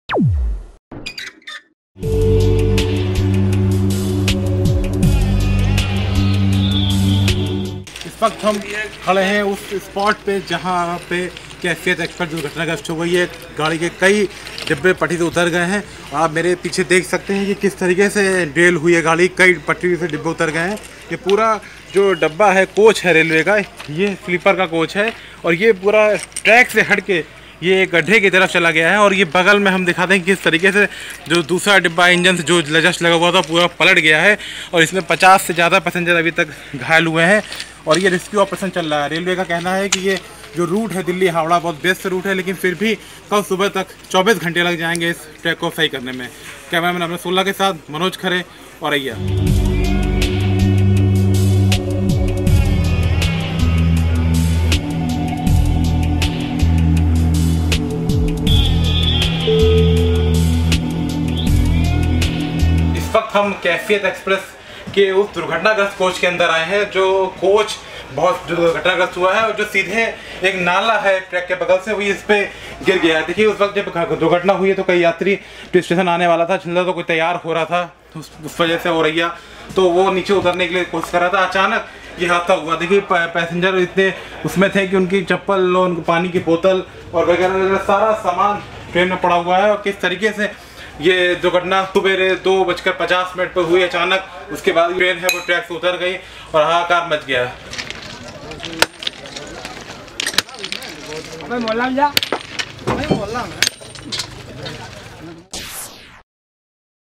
Kaifiyat Express’ 10 Coaches derail in Auraiya UP, over 100 passengers injured
यूपी के औरैया जिला स्थित अछल्दा और पाता रेलवे स्टेशन के बीच बुधवार को कैफियत एक्सप्रेस डंपर से टकराकर दुर्घटनाग्रस्त हो गई। घटना करीब सुबह तड़के 2.52 मिनट की बताई जा रही है। इस घटना में ट्रेन के करीब 10 डिब्बे पटरी से उतर गए। हादसे में करीब 100 से अधिक लोग घायल हैं। जिनमें चार की हालत नाजुक बताई जा रही है। हादसे के पीछे रेलवे स्टाफ की लापरवाही उजागर हो रही है।